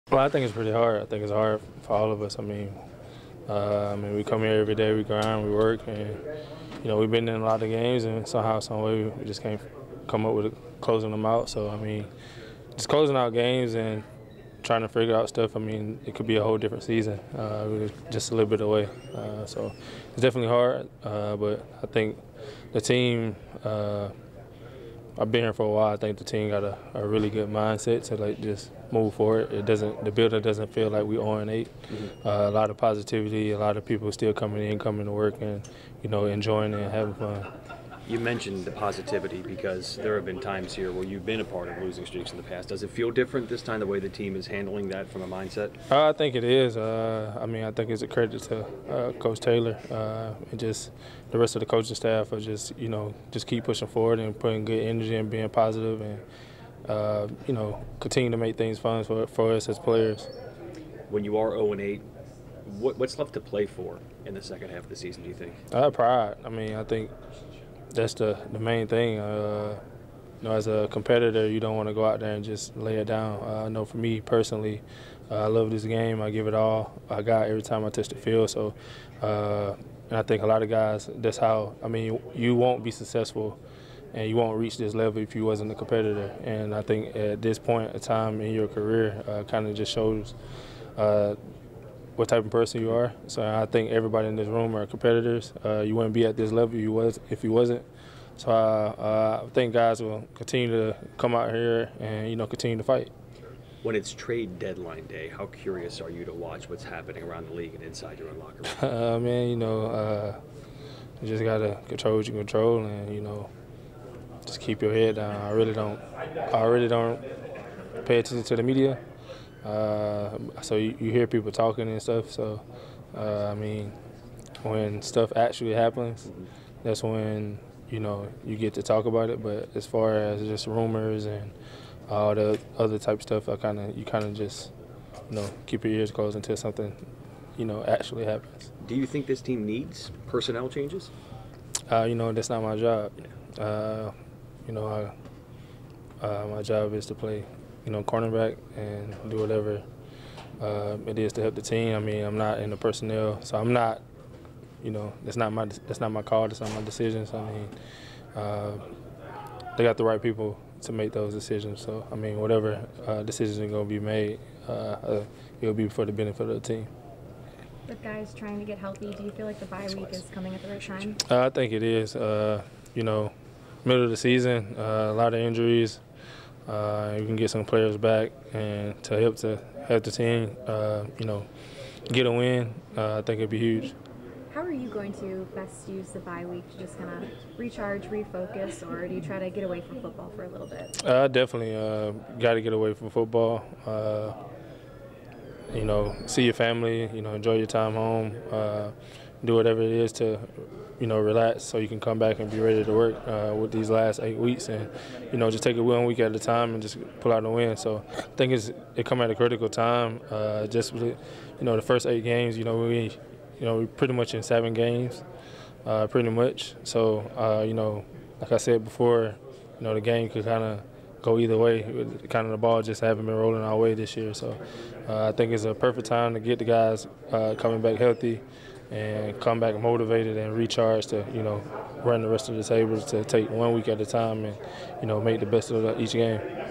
Inside The Locker Room: Darqueze Dennard - 10/29/19
Cornerback Darqueze Dennard chatted with media members about the team heading into the bye week.